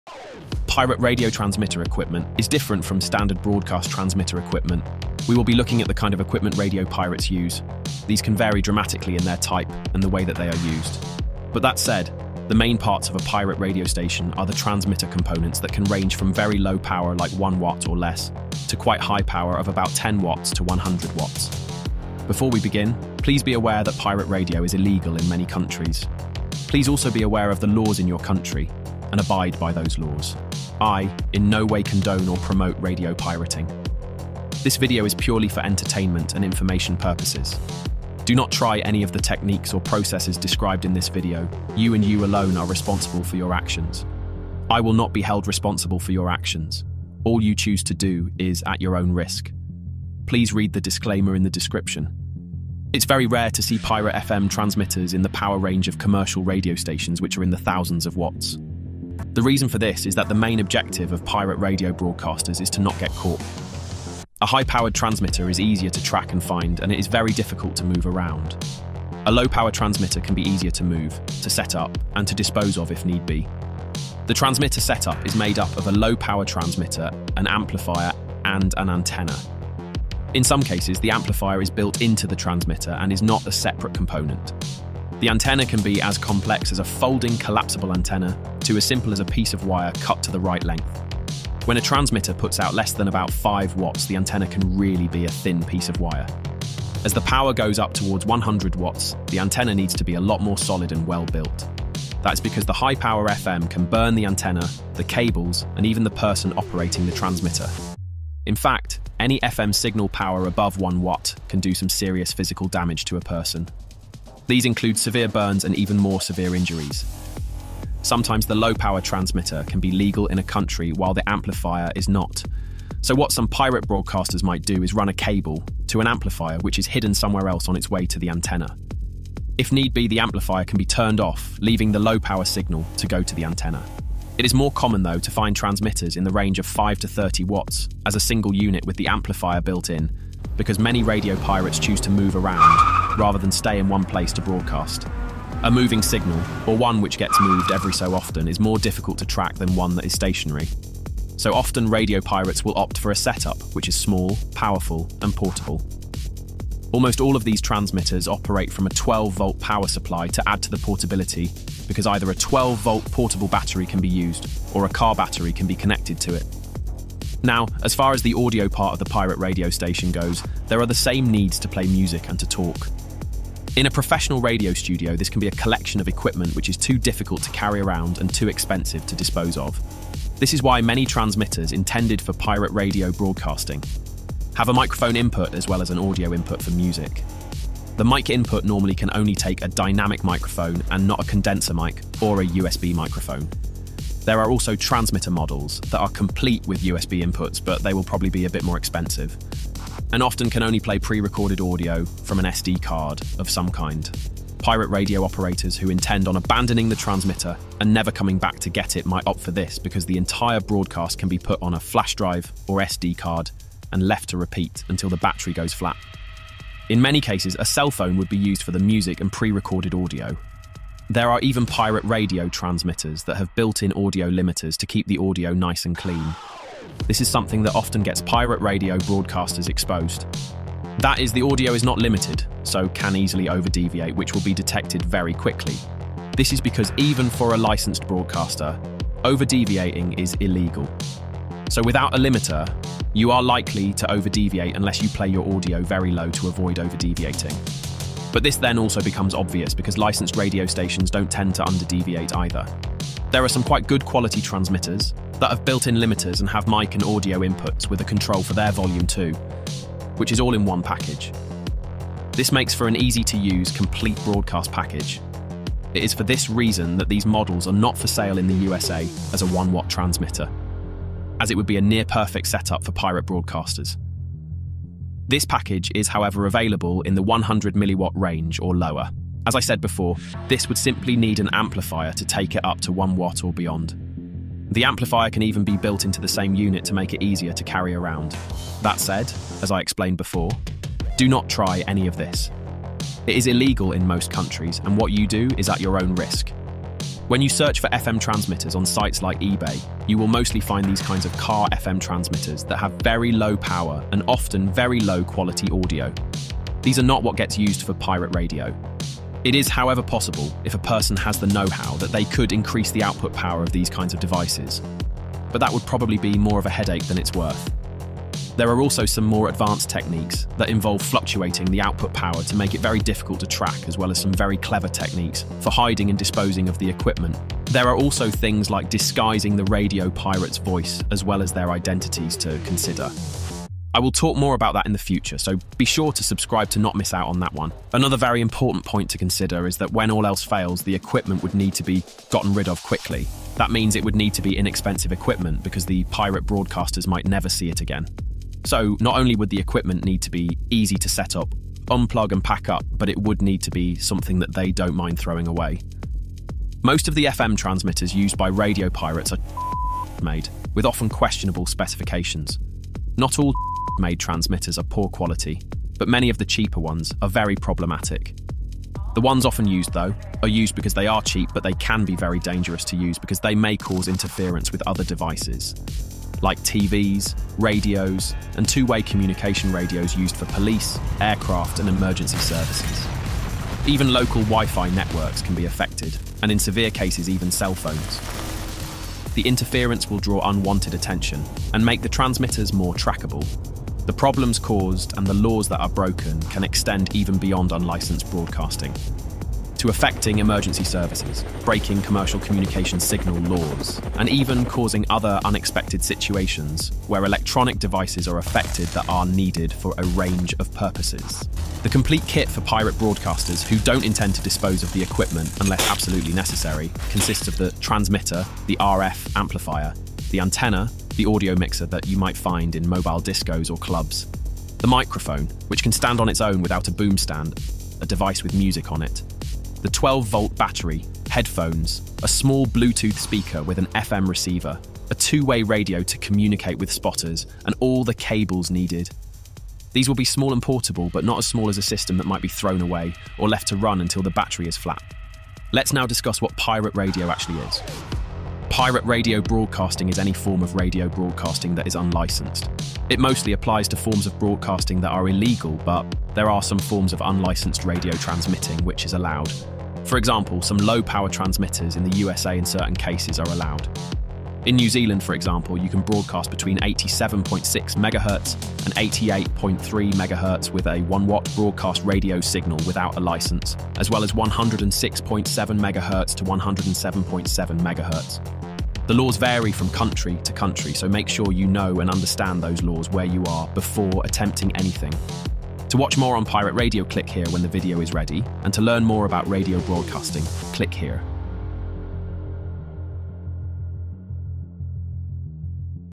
PIRATE Radio Station FM Transmitter Kit. Transmitter, Antenna, Audio setup home pirate radio station
Pirate radio station FM transmitters and all the audio and transmitter equipment needed to broadcast on FM without a license. Pirate radio broadcasting is illegal broadcasting on the FM and the AM broadcast band. There are legal forms of unlicensed broadcasting in some countries. These vary from country to country. Make sure you know, understand and adhere to the laws where you wish to broadcast.

In this video, we look at the equipment and techniques used by FM radio pirates. Pirate radio broadcasting is how people without a license transmit on the FM or AM broadcast band. The transmitter equipment, antenna, and audio equipment needed for a pirate radio station are different from those used for commercial FM broadcasts and even community radio broadcasts.